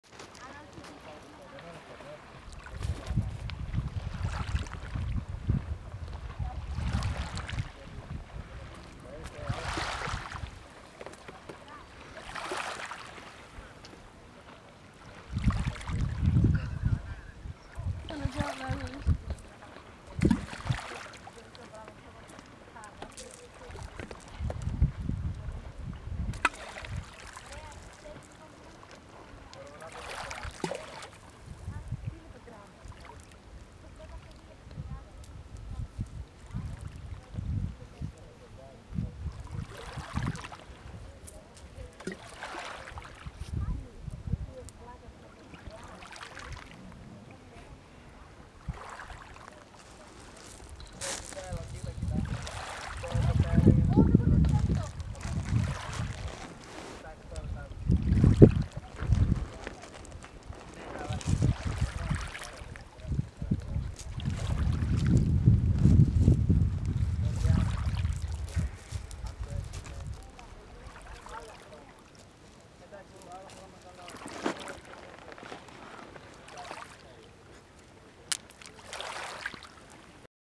Τοκμάκια ( οδοιπορικό στην Λέσβο )
εκδρομούλα στο Ασπρονήσι,ένα νησάκι από το σύμπλεγμα Τοκμάκια που βρίσκονται ανοιχτά του Μανταμάδου!